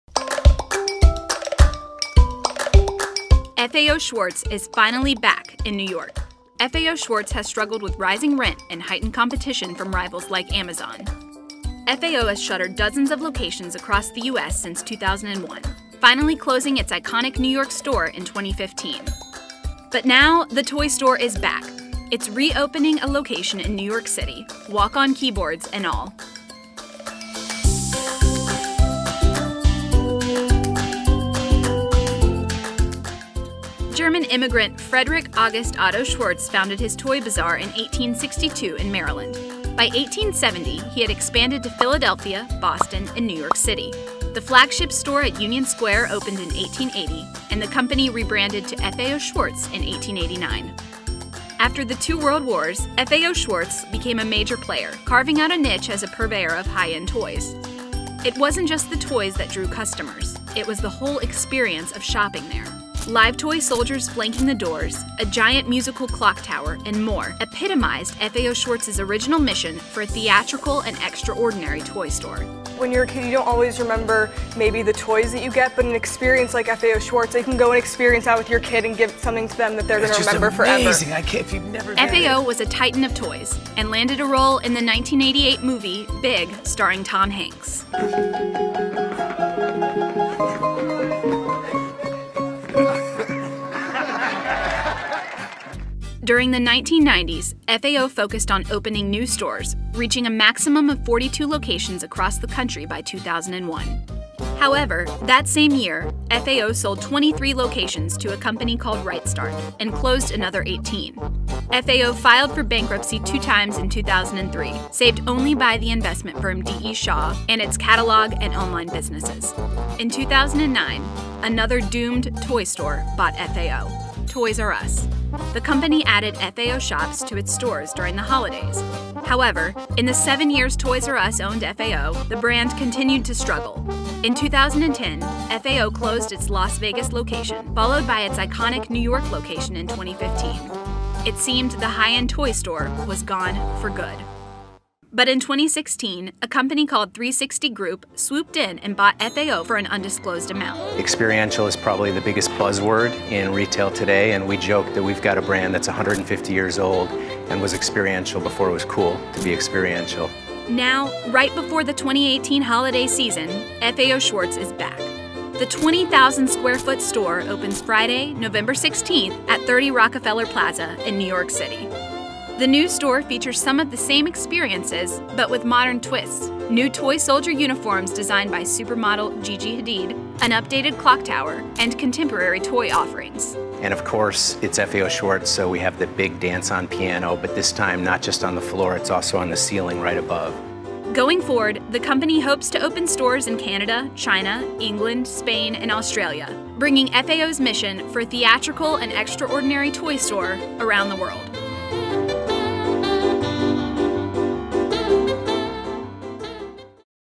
FAO Schwarz And Its Piano Are Back In NYC
Iconic toy retailer FAO Schwarz is making its return to New York City after shutting its famed flagship store on Fifth Avenue in 2015, when the brand was still owned by Toys R Us.

Now under a new owner, FAO Schwarz will bring its plush stuffed animals and walk-on piano keyboard — featured in the 1988 movie "Big" with Tom Hanks — back to life, and just in time for the 2018 holiday season. The store opens Friday at 30 Rockefeller Plaza in Manhattan. FAO Schwarz, meanwhile, has already embarked on expanding around the globe.

The goal is to make the space "experiential" and one that can "deliver theater and can drive customers," David Conn, CEO of FAO Schwarz's parent company ThreeSixty Group, told CNBC last month.

Brands featured in the 20,000-square-foot 30 Rock store include two of those also owned by ThreeSixty — electronics from The Sharper Image and toys from Melissa & Doug. There are walls of Barbie dolls, a Build-A-Bear Workshop station, a racetrack for kids to play with cars, a space where a magician will teach tricks, a candy shop and so much more for visitors to do there, beyond shopping.

And, of course, FAO Schwarz brought back its iconic keyboard on the floor. (This time it's also on the ceiling, in bright lights, to spark the curiosity of those walking by.) The massive clock tower is there too, along with a new centerpiece, a rocket ship.

Without Toys R Us now that it's bankrupt, there is a huge share of the toy market left up for grabs, giving FAO Schwarz the chance to make a name for itself again. FAO Schwarz will also be selling its own branded items inside other retailers including Kohl's and Hudson's Bay this holiday season.

Back at 30 Rock, FAO Schwarz will offer everything from hot items like Hatchimals to the brands older generations of consumers grew up with, like Steiff teddy bears. Its goal is to be a store that customers remember and keep coming back to.

The experience is the key. It's a strategy many retailers are employing to lure consumers out of their homes and into the stores — but it's also part of FAO Schwarz's heritage. The fun of going to the old store is a memory for many generations, but today's shoppers may find many "Instagrammable" moments inside the walls of the new store.

Inside FAO Schwarz's New 30 Rock Store